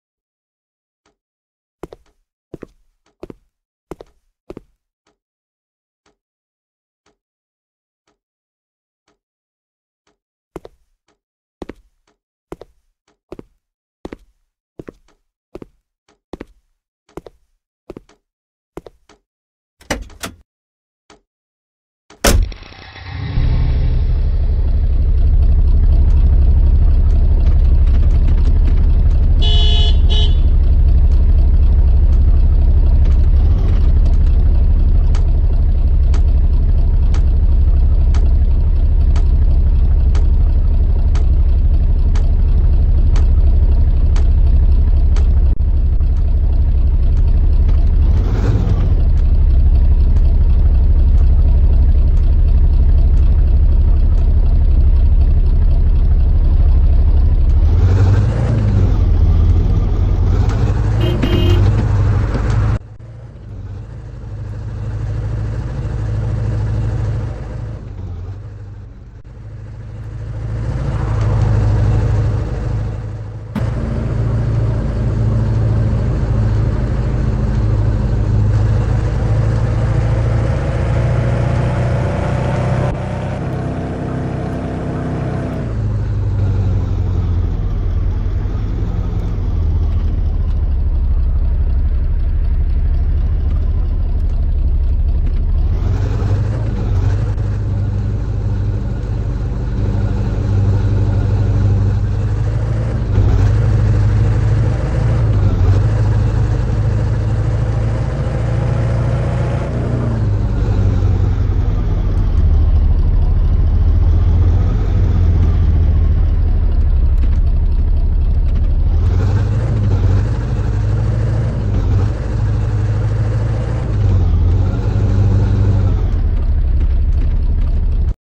Toyota Innova car driving game - gadi wala game - realistic car game android gameplay
In this video i am playing Toyota Innova car driving game - gadi wala game - realistic car game android gameplay. i love to play this game. QK es game k graphic and gameplay bahot hin jabr10 hai ager ap k es game ko try krna chaty ho to google play store sy es game ko download karen.